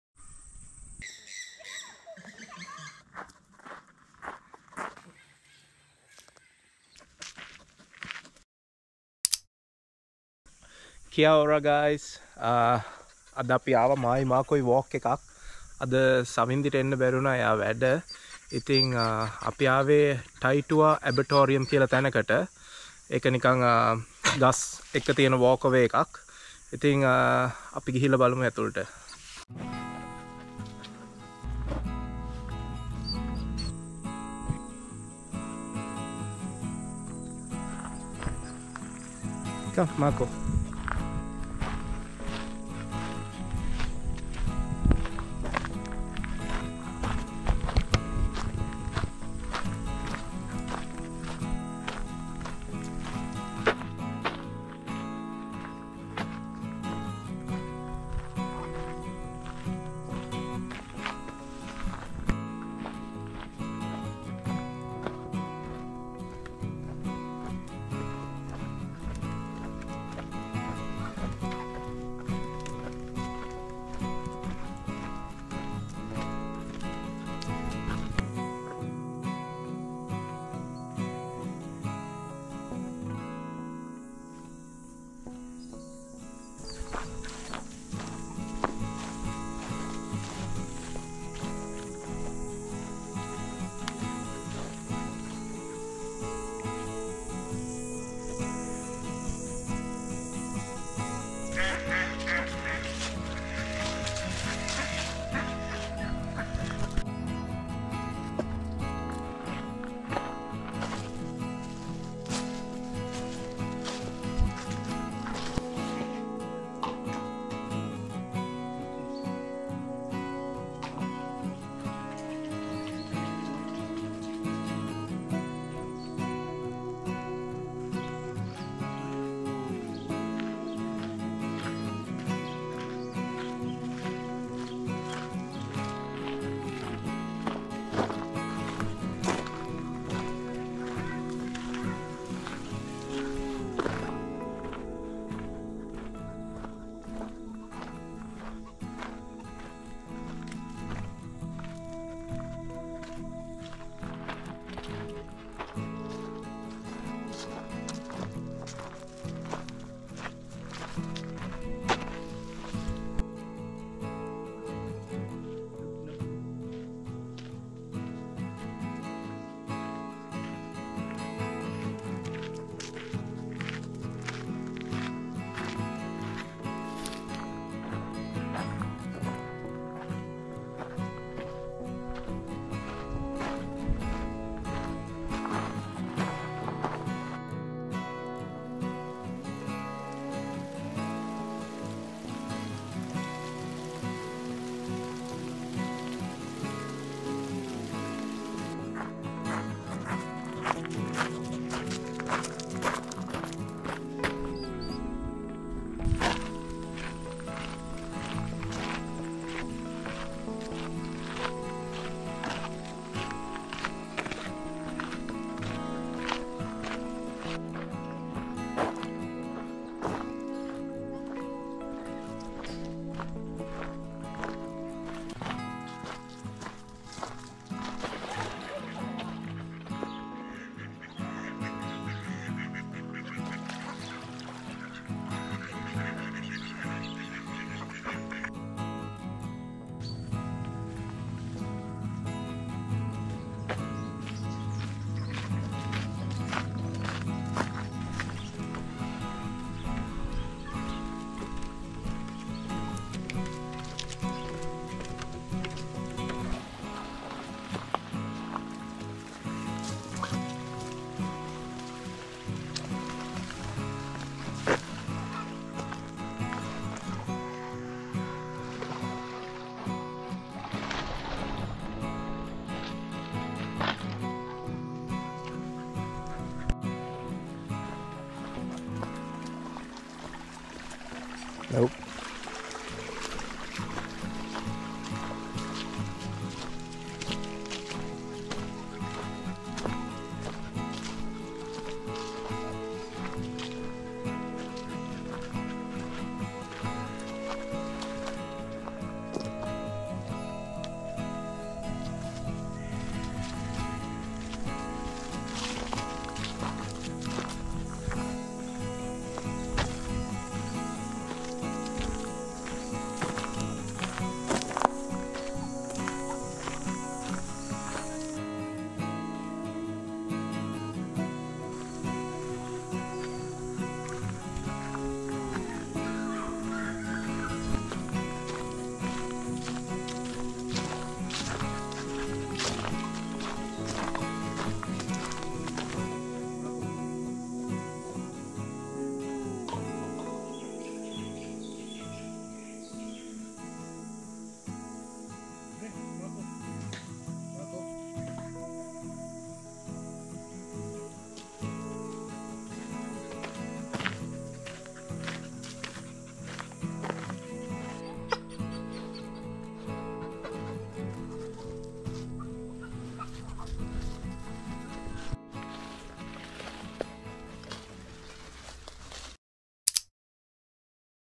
Exploring Taitua Arboretum: A Tranquil Journey Through Hamilton's Botanical Paradis | Sinhala vlog 4
Taitua Arboretum in Hamilton, New Zealand, is a hidden gem for nature lovers and outdoor enthusiasts. This expansive botanical garden features an impressive collection of both native and exotic trees, creating a rich tapestry of colors and textures throughout the seasons. As you explore the arboretum, you'll encounter winding trails, charming bridges, and peaceful spots to relax and enjoy the natural beauty surrounding you. Whether you're interested in botanical photography, leisurely walks, or simply reconnecting with nature, Taitua Arboretum offers a perfect setting for a peaceful experience.